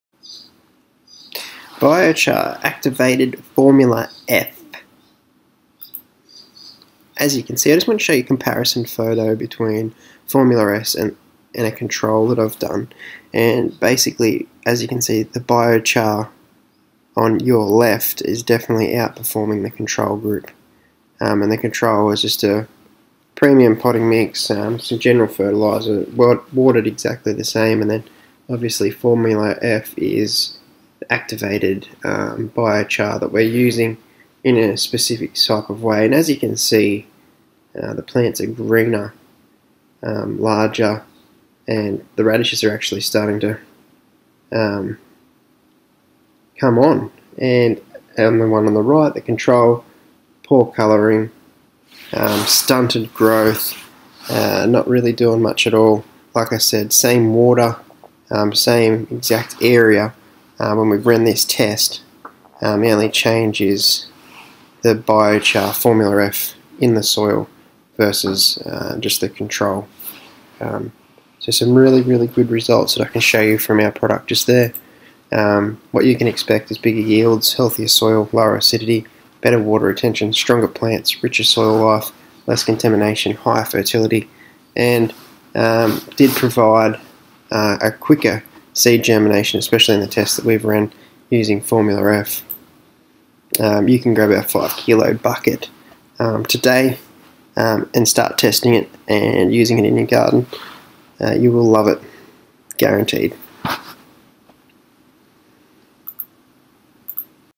Biochar Formula F 5 kg Bucket
The Ultimate Way To Increase Yields By At Least 25%.
I read the hype but I didn’t believe it.
So I had to test it.
They say a picture is worth a 1000 words.
I have tested Biochar extensively and every time it comes up trumps against every other fertiliser compost variation.
The picture above is the best formula I have found so far.
So if you want:-
Bigger yields with less effort formula F is the answer.
Healthier soil.
Biochar builds your soil food web so you can do back to back harvests.
Lower acidity. Biochar can stay in the soil for 1000 of years.
Better water retention works like a vessel in soil that the plants can draw upon in times of need.
Stronger plants. Plants can handle hotter and colder weather. You may be able to grow out of the regular season.
Richer soil life. One gram of Biochar folded out has the surface area of two tennis courts.
Less contamination absorbs toxins in soil and stores them away.
Higher fertility.  Makes nutrients more available for plants.
Promotes seed germination.  In every test I have run, seeds germinate an average of 25% faster.
Biochar is the one product you can add to your soil that will just keep giving.
Get your garden what it needs to succeed today.